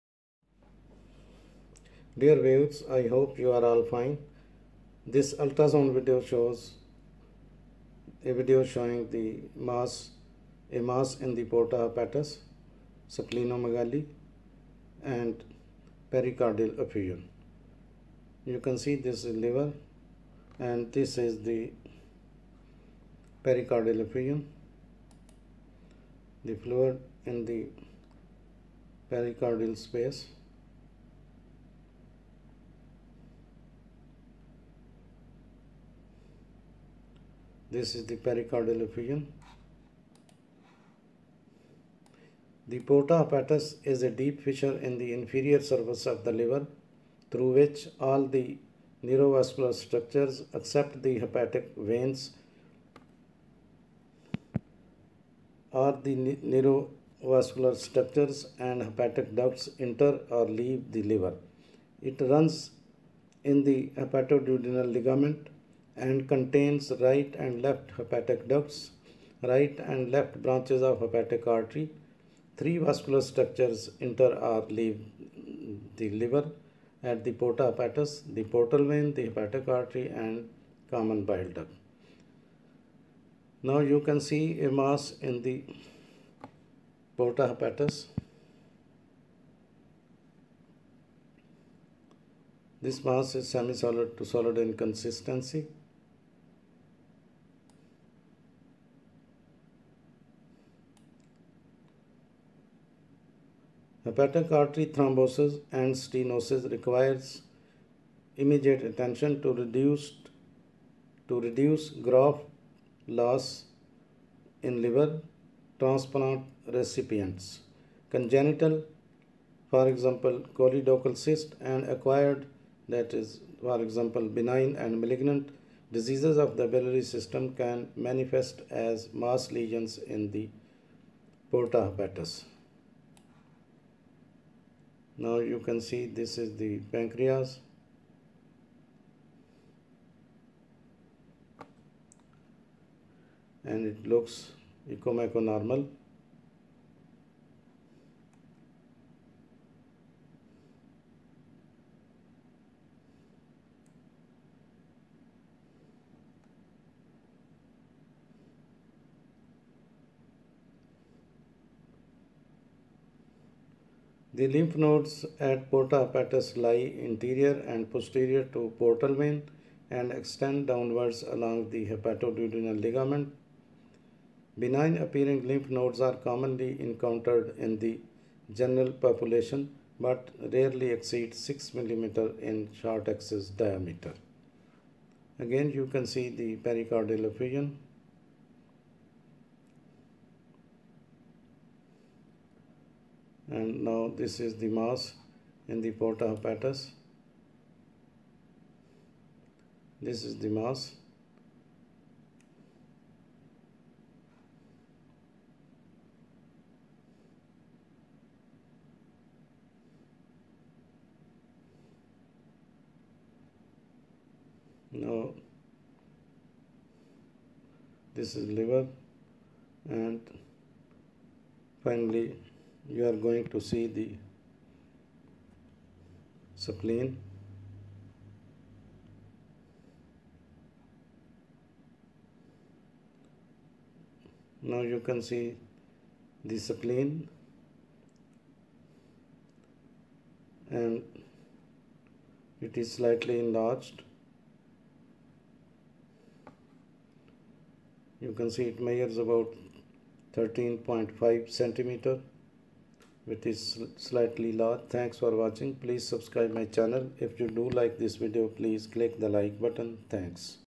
Ultrasound Video showing Mass in Porta hepatis, Pericardial effusion, and Splenomegaly.
This video shows Mass in Porta hepatis, Pericardial effusion, and Splenomegaly.
The porta hepatis is a deep fissure in the inferior surface of the liver through which all the neurovascular structures (except hepatic veins) and hepatic ducts enter or leave the liver. It runs in the hepatoduodenal ligament and contains: right and left hepatic ducts. right and left branches of the hepatic artery. Three vascular structures enter or leave the liver at the porta hepatis: the portal vein, the hepatic artery, and the common hepatic duct.
Hepatic artery thrombosis and stenosis require immediate attention to reduce graft loss in liver transplant recipients. Congenital (e.g., choledochal cyst) and acquired (benign and malignant) diseases of the biliary system can manifest as mass lesions in the porta hepatis.
The lymph nodes at the porta hepatis lie anterior and posterior to the portal vein and extend down along the hepatoduodenal ligament. Benign-appearing lymph nodes are commonly encountered in the general population but rarely exceed 6 mm in short-axis diameter.
Small pericardial effusions are often occult on plain film. Greater than 200 mL of pericardial fluid is usually required to become radiographically visible. Radiographic signs include: there can be globular enlargement of the cardiac shadow giving a water bottle configuration.
                         Difference between pericardial effusion and cardiac tamponade
Because of the limited amount of space in the pericardial cavity, fluid accumulation leads to increased intra-pericardial pressure which can negatively affect heart function. A pericardial effusion with enough pressure to adversely affect heart function is called cardiac tamponade.
Fetal pericardial effusions (FPE) occur when there is an accumulation of pericardial fluid in utero. In order to be considered as abnormal, it is generally accepted that the pericardial fluid thickness should be greater than 2 mm.